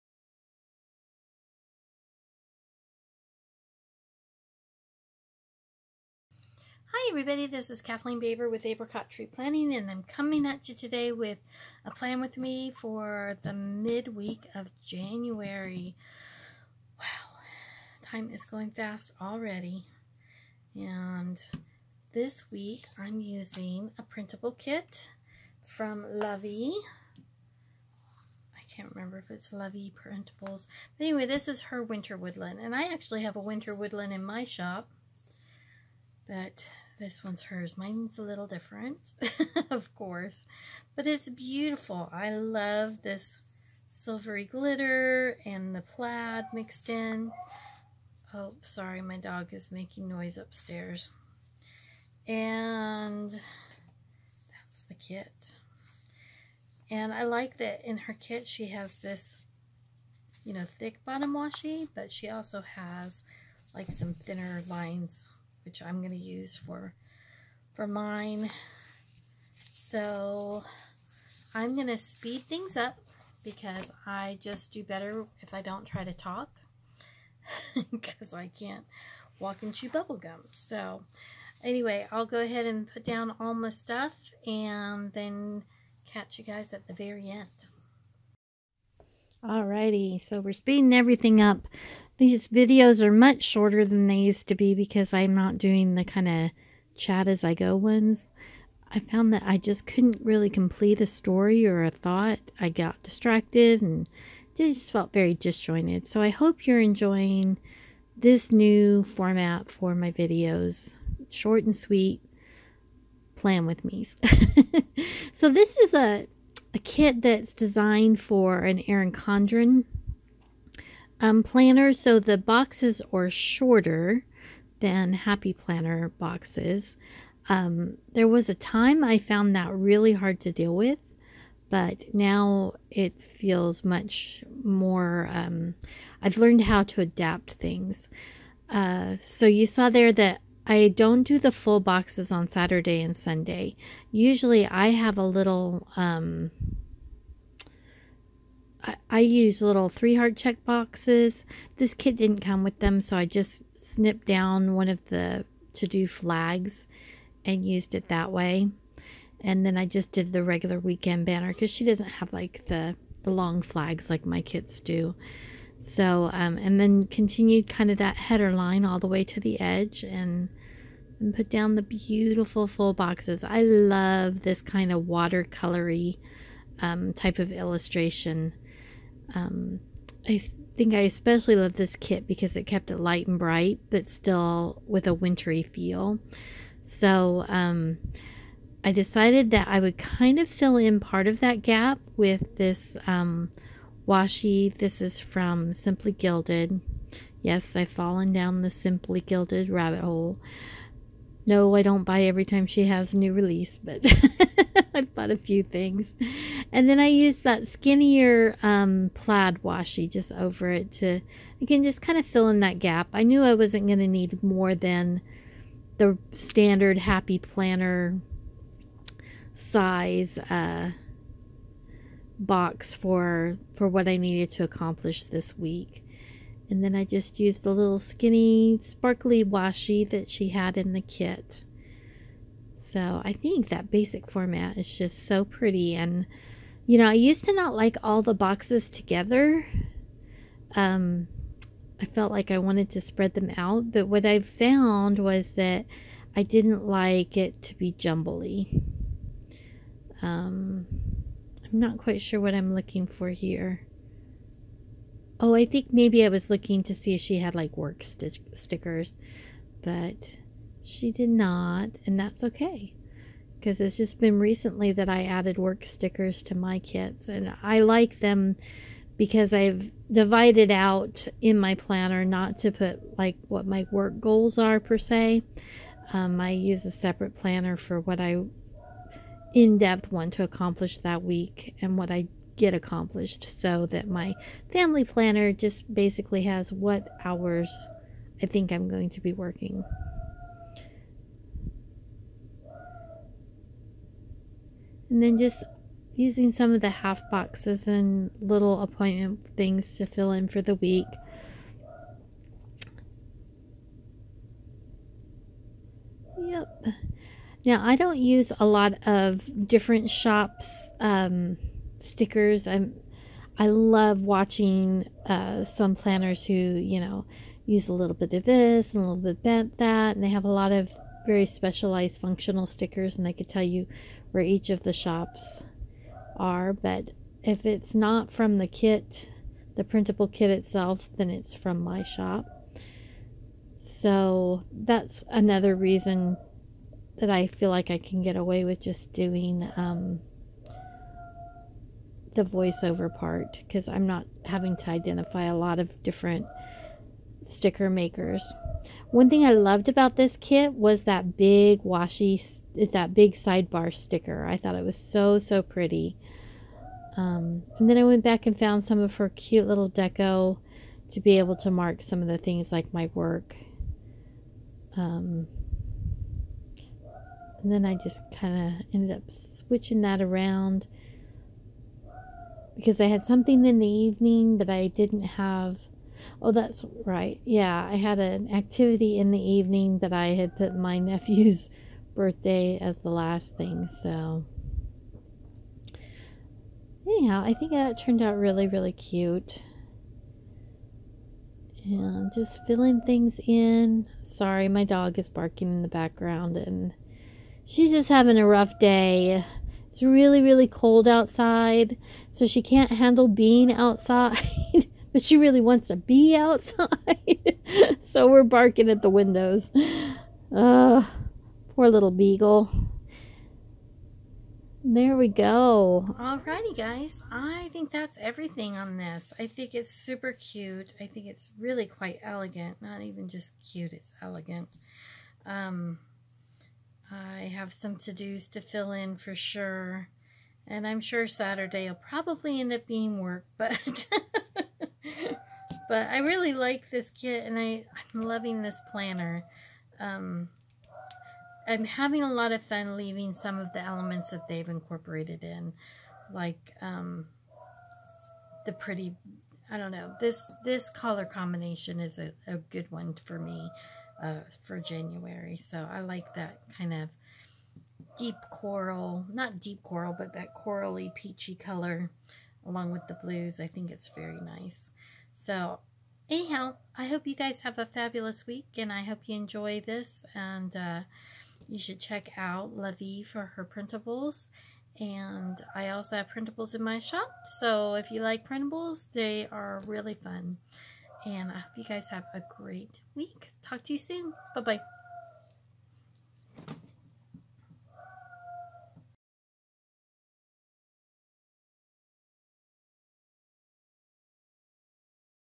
Plan With Me: Jan 14-20, 2019 | Winter Woodland by La Vie Prints
Thanks for stopping by my channel.  I hope you'll enjoy this quick plan with me.  I used a beautiful kit from La Vie Prints.